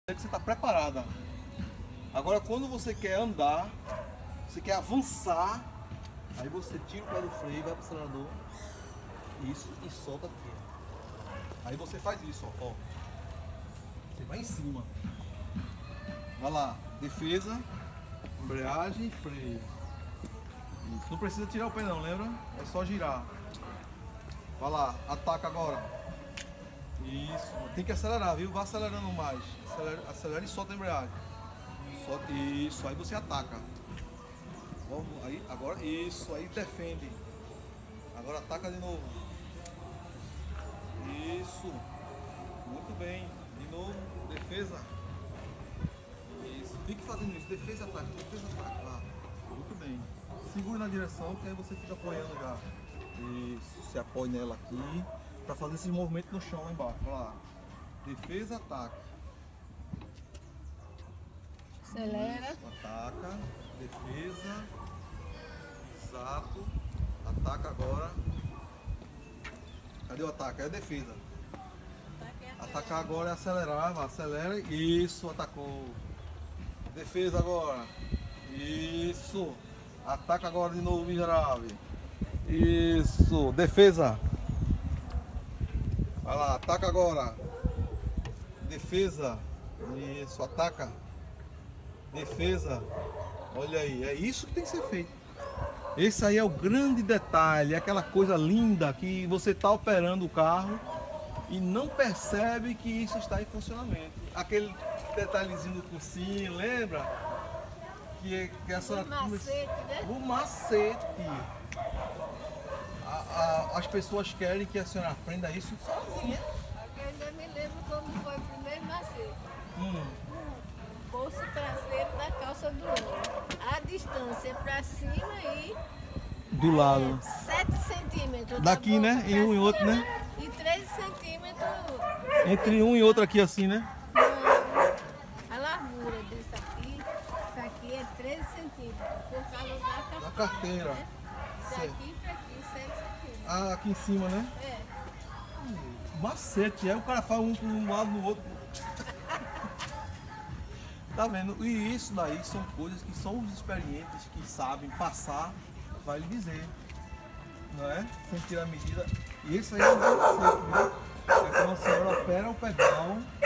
Como dominar o carro- SOS Dirigir
E quem disse que é tarde pra começar? Olha que coisa mais linda, D. Josefa com 70 anos, iniciando nesse momento sua libertação.  E você tá esperando o que pra começar?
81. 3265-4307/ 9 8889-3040